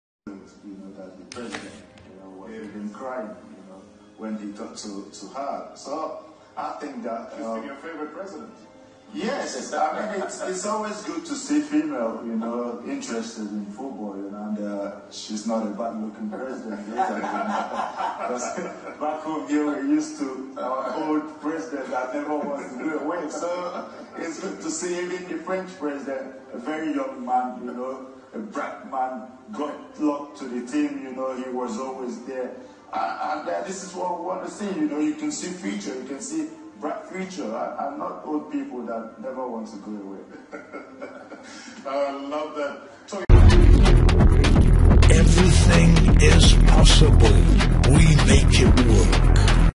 Okocha hits Buhari as Croatia, France presidents attend 2018 World Cup final VIDEO
Former Super Eagles captain, Austin ‘Jay Jay’ Okocha, took a shot at President Muhammadu Buhari, after the 2018 World Cup final between Croatia and France on Sunday.

Both teams received cheers in Moscow by their respective presidents, Kolinda Grabar-Kitarovic and Emmanuel Macron.

Okocha, speaking on SuperSport, said Nigeria was used to presidents who never want to go away.

“You see the Croatian President, not a bad looking one. And the French President too, a young man who clicked with the team all along not some old man who doesn’t want to go away,” he said.

Grabar-Kitarovic became a huge fans’ favourite throughout the tournament, after taking an unpaid leave to support her national team in Russia.

Source:: NairaLearn TV

University Of NairaLearn